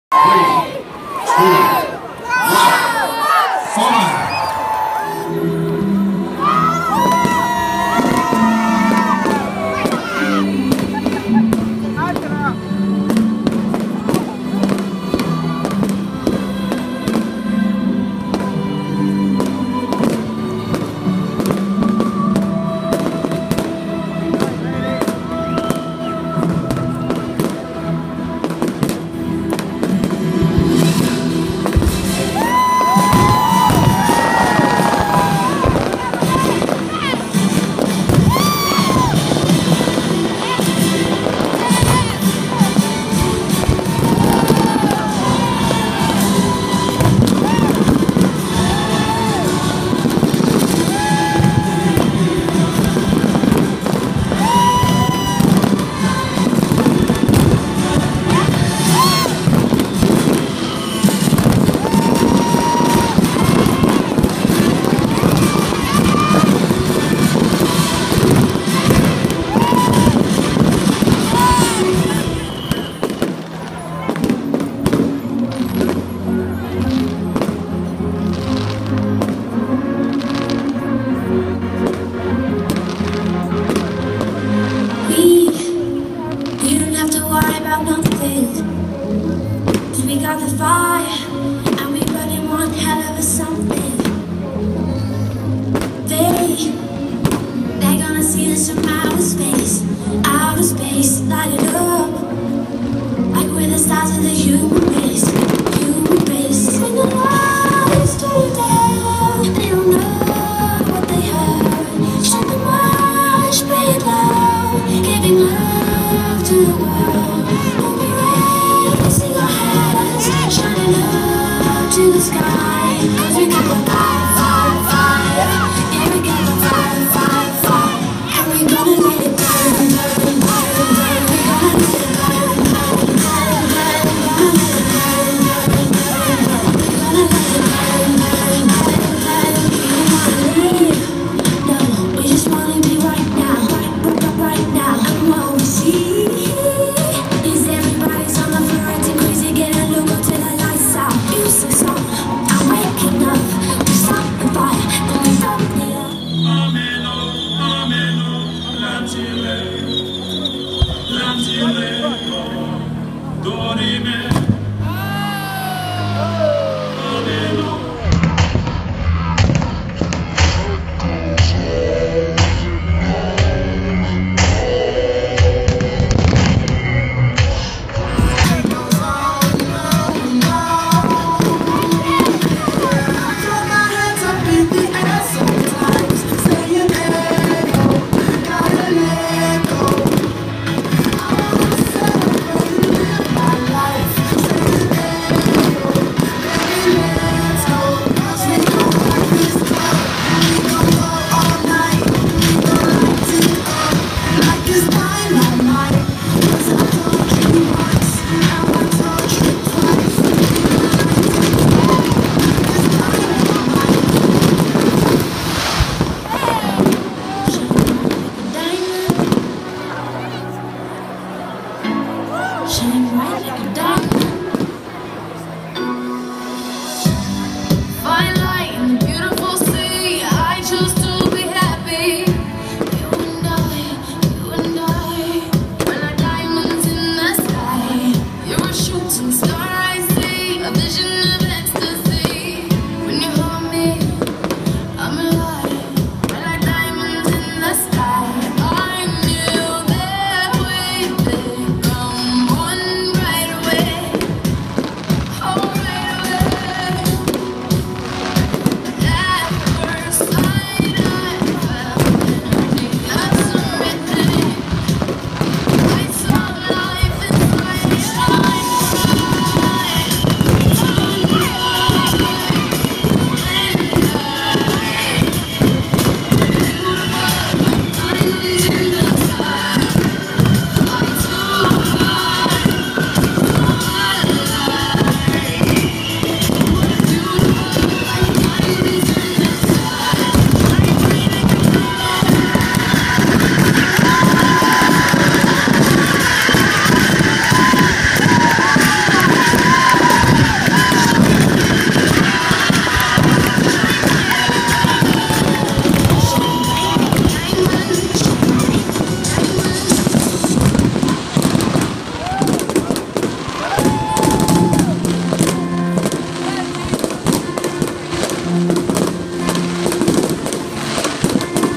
Ascot Race course spectacular fireworks show
A winter's evening firework show at Ascot races.  Enjoy the fantastic firework show close up on the running rail. Singalong to the songs and feast your eyes on the light show. Filmed by yardski